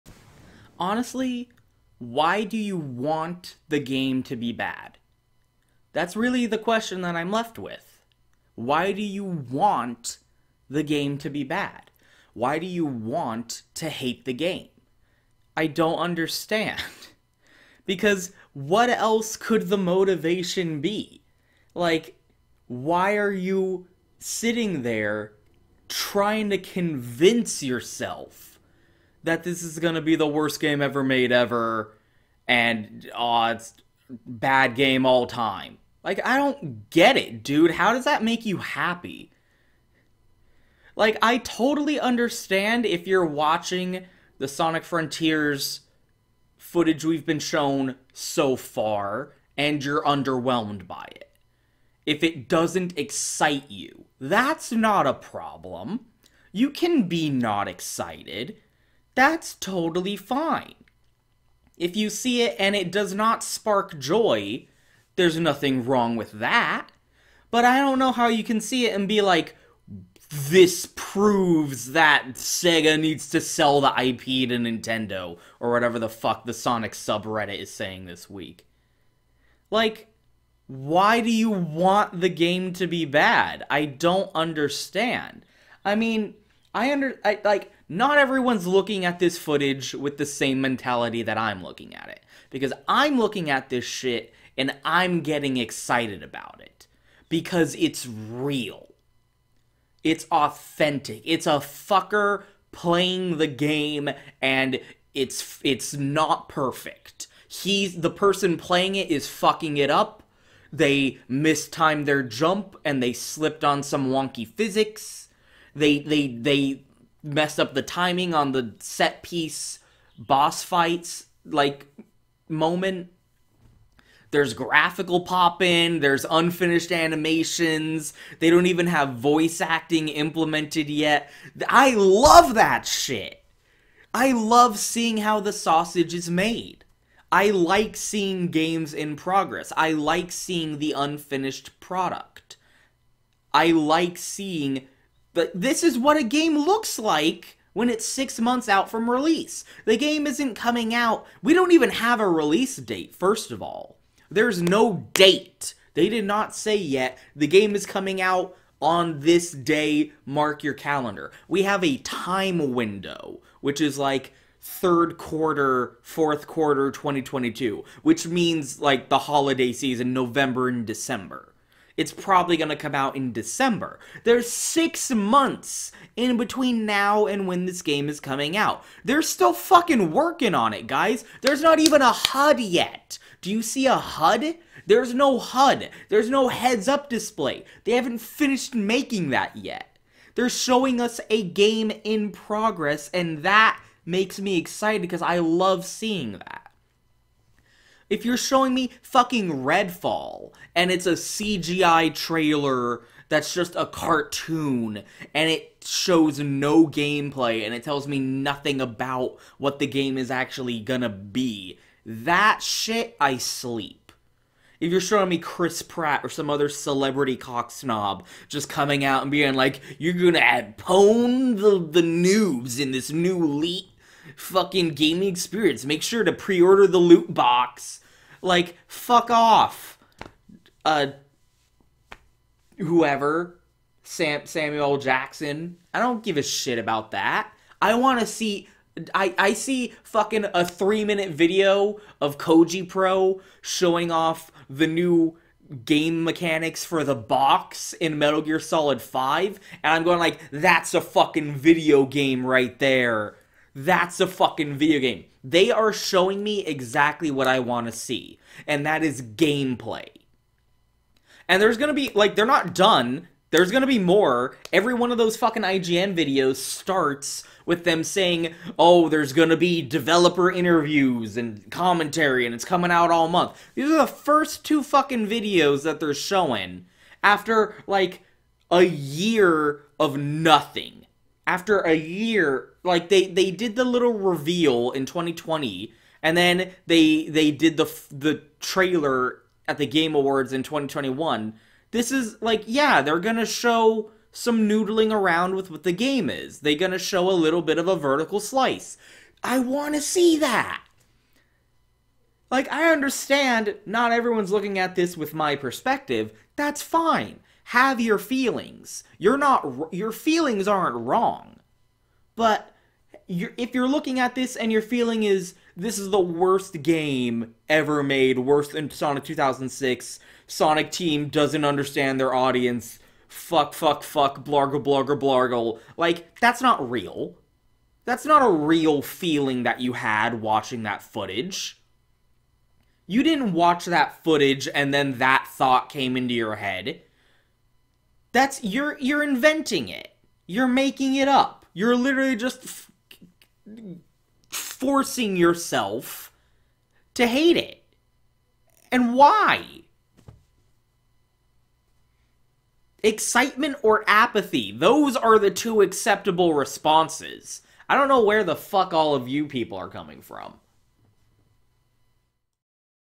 Foxeh Reacts to Sonic Frontiers Reactions
Is it unfair to call the sonic hatedom a cult at this point? I don't know what else to call it.